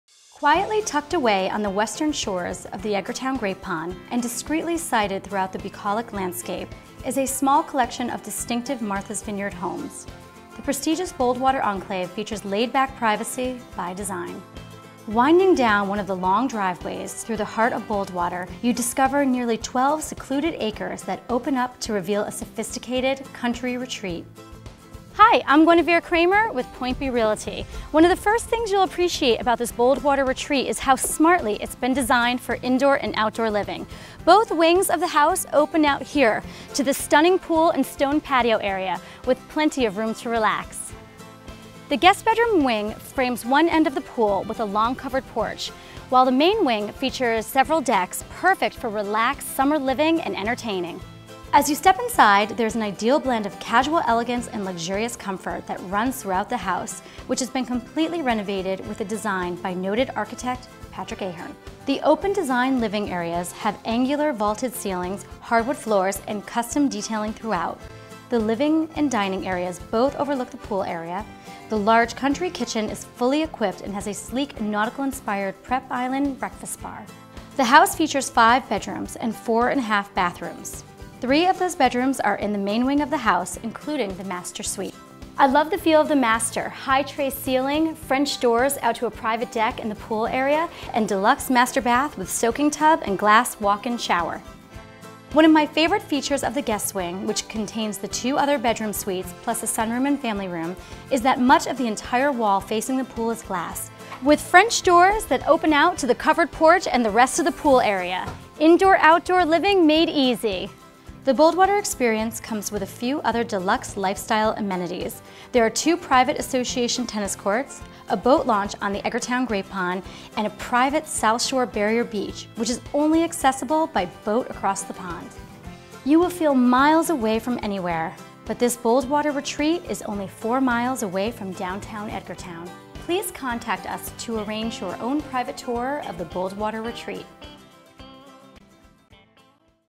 8 Boldwater Road Edgartown MA 02539 Martha's Vineyard Real Estate
Sophisticated Country Retreat On Martha's Vineyard - Set amid nearly 12 pastoral acres in the exclusive enclave of Boldwater, this sophisticated country retreat blends casual elegance and luxurious comforts within a beautiful and private setting. Fully renovated pursuant to designs by noted architect, Patrick Ahearn, the home perfectly integrates indoor and outdoor living around a heated gunite pool with bluestone terrace and beautifully landscaped grounds and gardens. The open-designed living areas feature vaulted ceilings, hardwood floors and custom details throughout. There are a total of five bedrooms, including a spacious first-floor Master Suite and an attached two-bedroom Guest Wing, which is ideal for family and guests. Access to all Boldwater association amenities -- including tennis courts, Edgartown Great Pond boating access and a private South Shore beach -- is included in this unique offering.